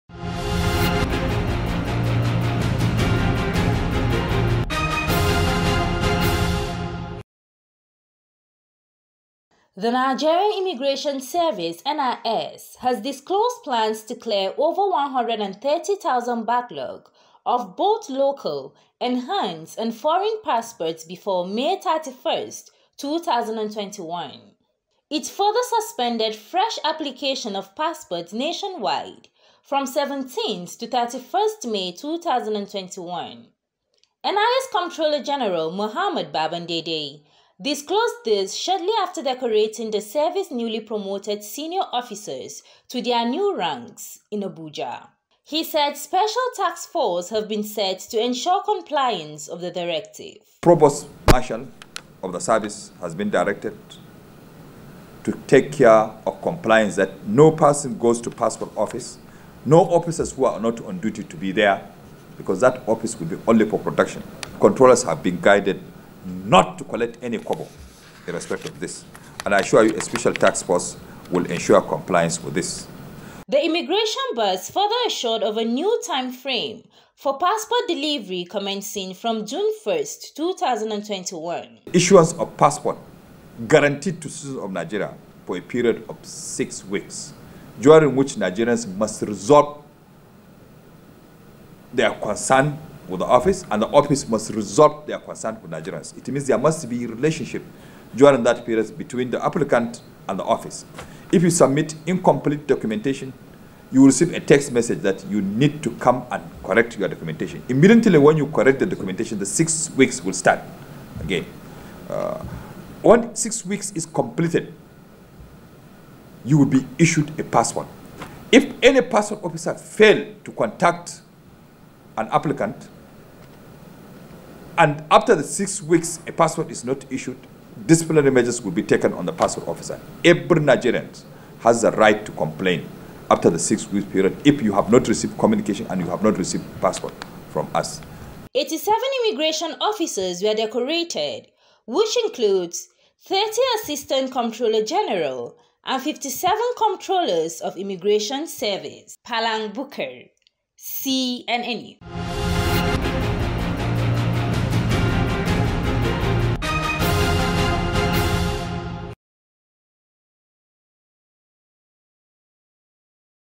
NIS Suspends Fresh Passport Application, Sets Deadline To Clear Backlog
The Nigerian Immigration Service NIS has disclosed plans to clear over 130,000 backlog of both local enhanced and foreign passports before May 31st 2021.

It further suspended fresh application of passport nation wide  from 17th to 31st May 2021.

NIS Comptroller General Muhammad Babandede disclosed this shortly after decorating the service newly promoted senior officers to their new ranks in Abuja.

He said special task force have been set to ensure compliance of the directive .

Track up

 The Immigration boss further assured of a new timeframe for passport delivery commencing from June 1st 2021.

Track up

Eighty seven Immigration officers were decorated which include, thirty Assistant Comptroller General and fifty seven Comptrollers.